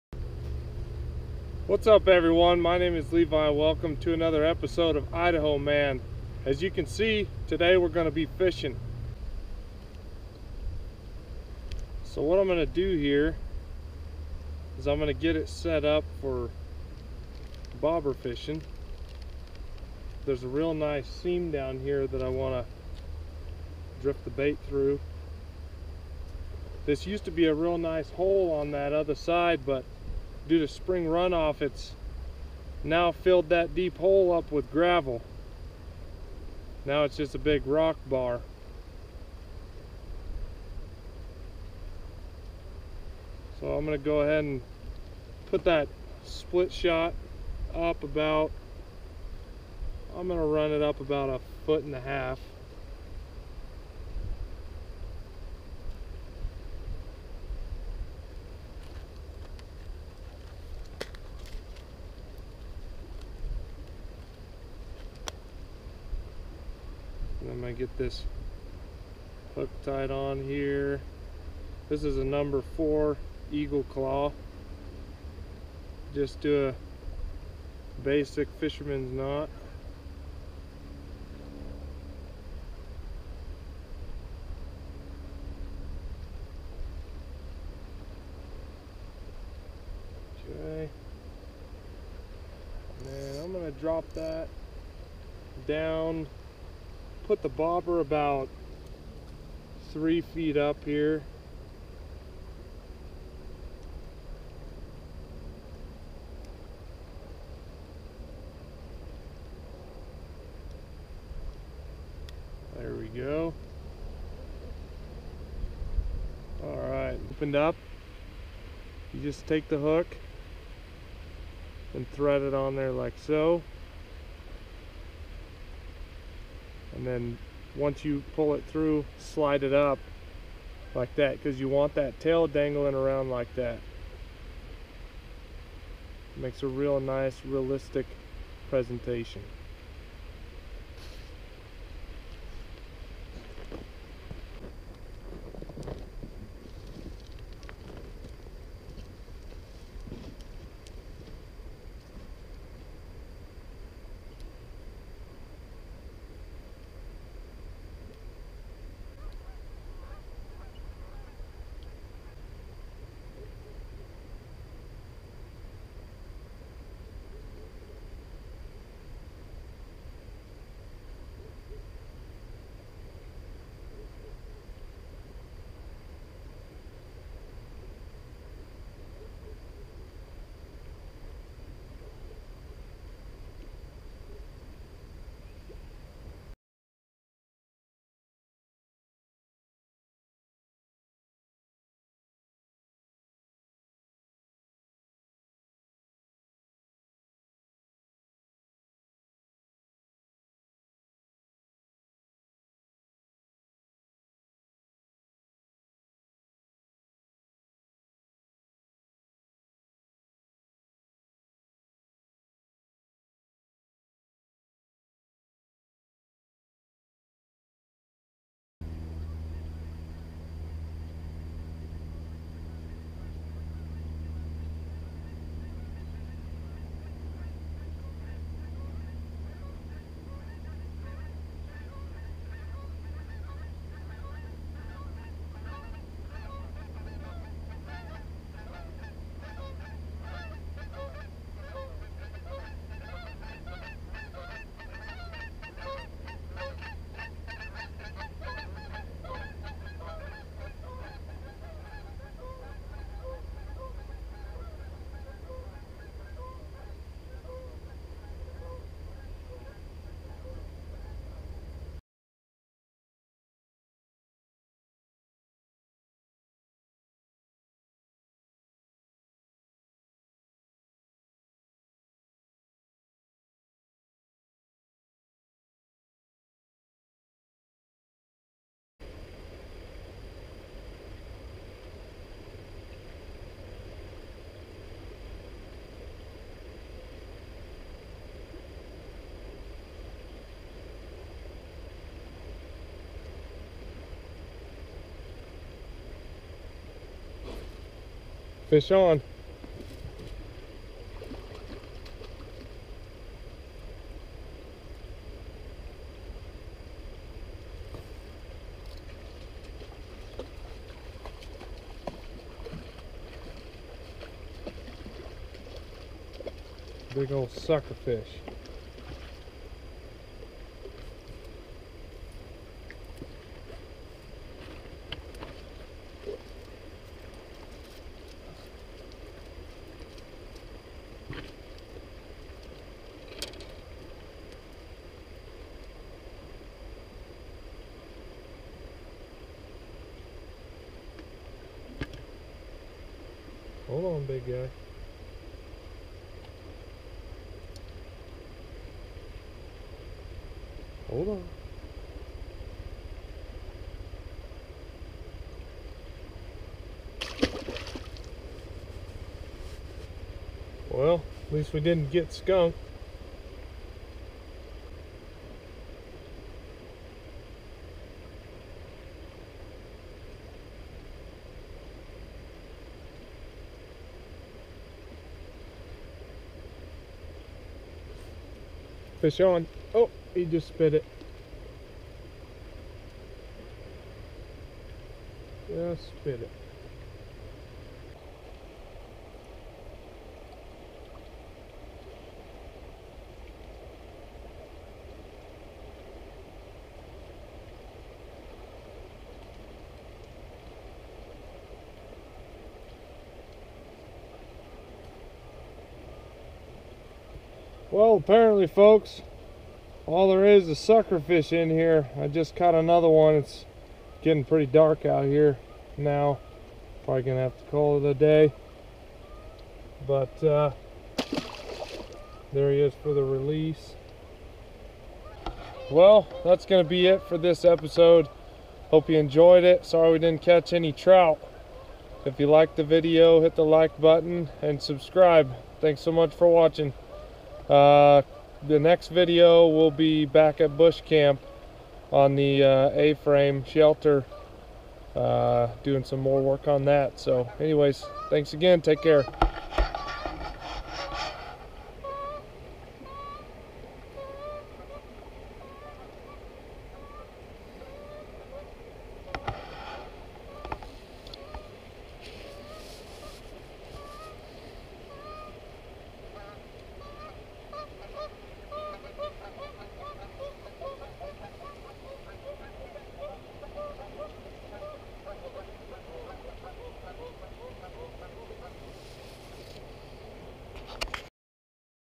Trout Fishing | Turns To Large Sucker Fish
It was a nice day so I headed down to the river to try and catch a trout. After many hours of trying different spots and methods I came up empty. so for the last little bit of daylight I let the worm set on the bottom.